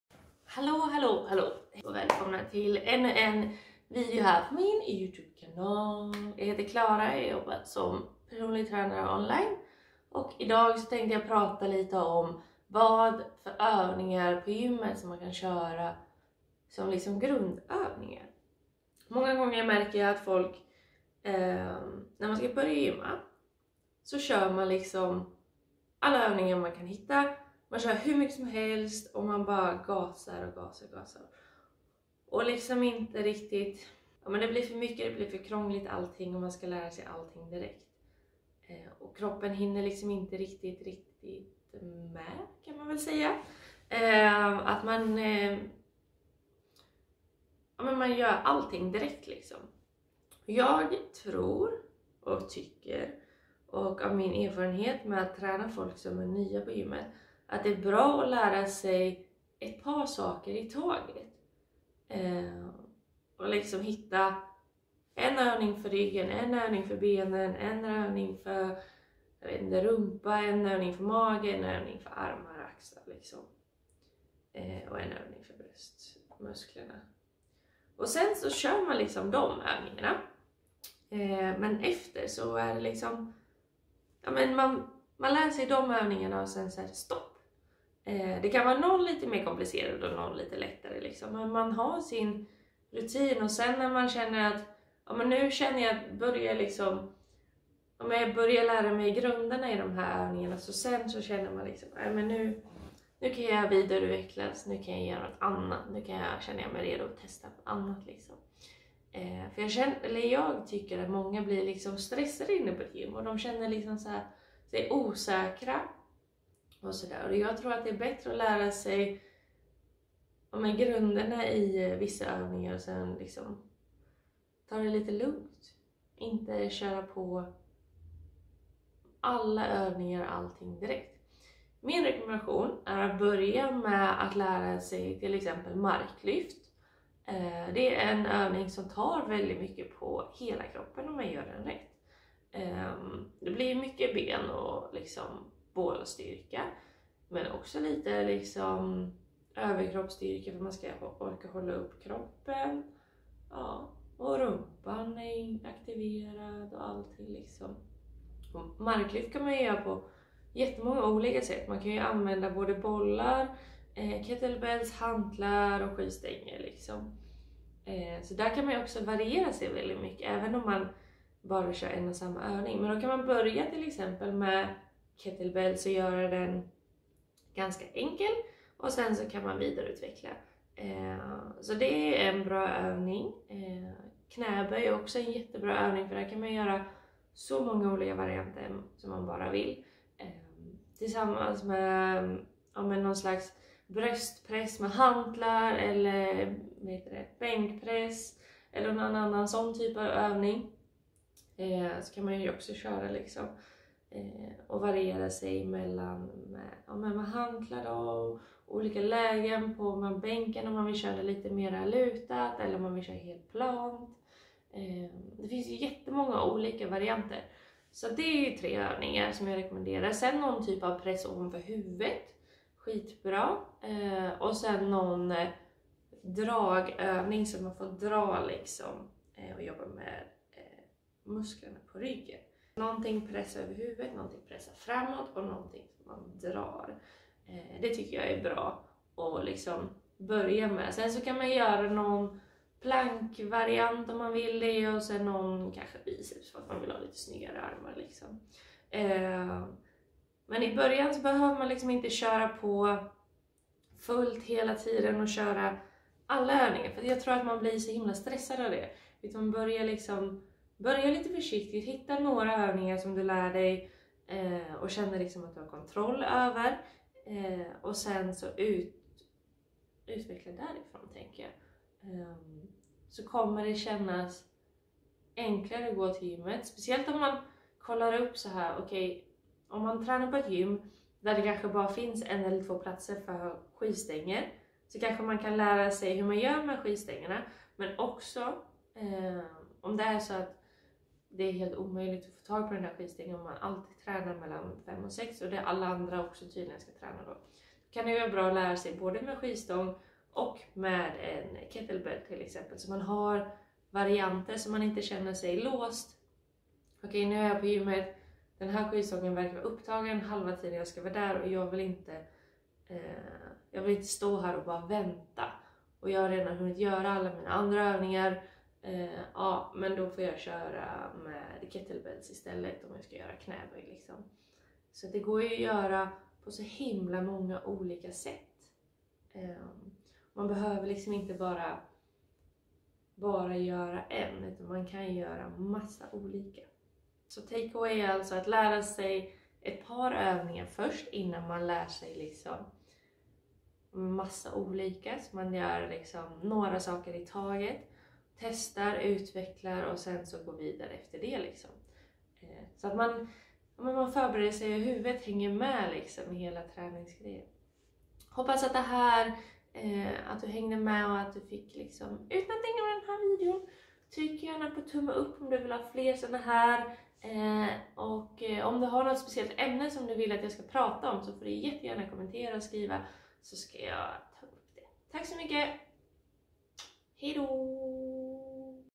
COFFEETALK | Börja gymma - de här övningarna är bra att börja med
HEJ!

Välkommen tillbaka till coffeetalks!

Idag tipsar jag om de bästa nybörjarövningarna att satsa på i gymmet!

Personlig tränare och influencer på Clara Färdiga Gå